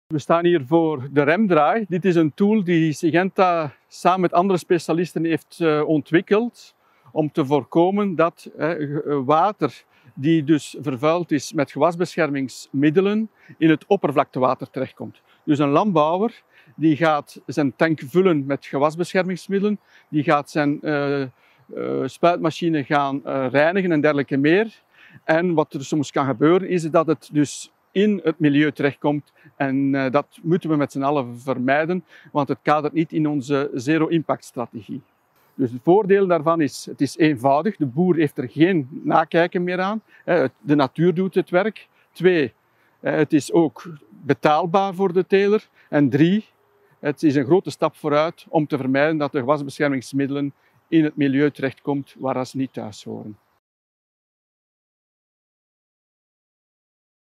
Interra Remdry
Kijk mee hoe we op onze Interra boerderij in Ittre (nabij Brussel) duurzame landbouw in de praktijk brengen. In deze video uitleg over de Remdry: een eenvoudig systeem waarmee wordt gezorgd dat resten gewasbescherming niet in het milieu terechtkomt.